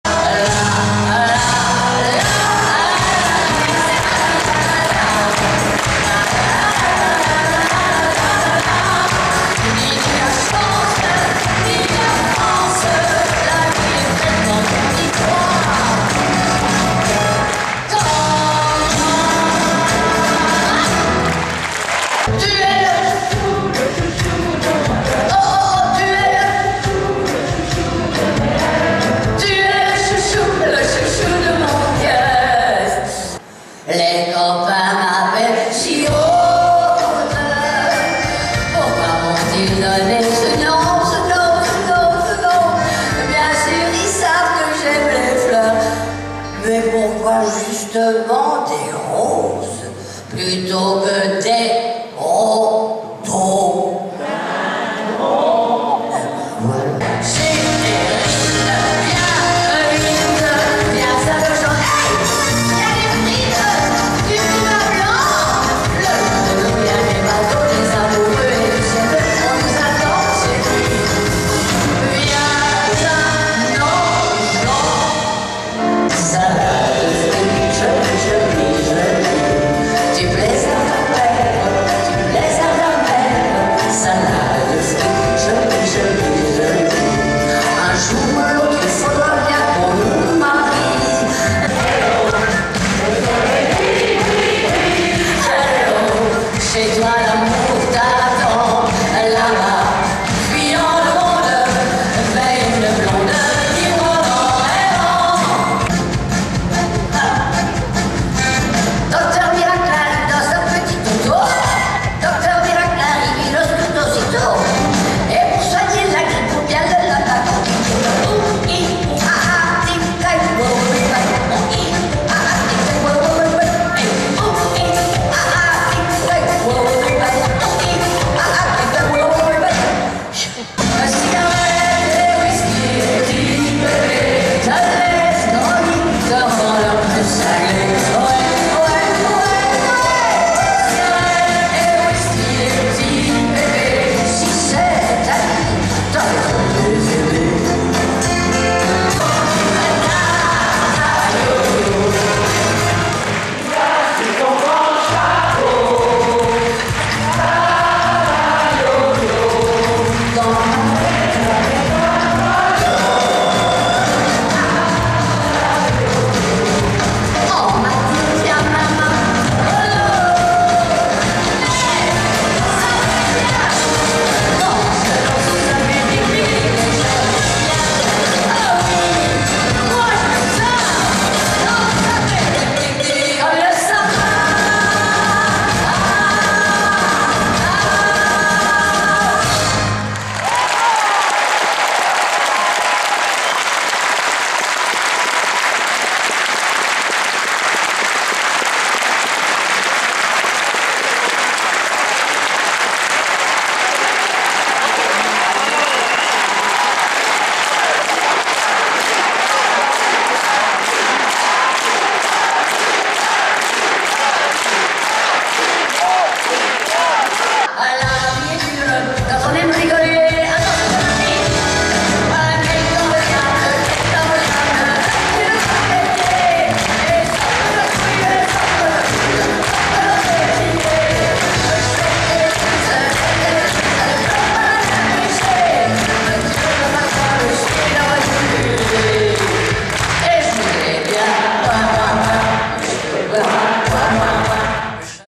age tendre et tete de bois Saison 6 Annie Cordy
Annie Cordy Palais des Congrès Dimanche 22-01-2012 Spectacle de 19h45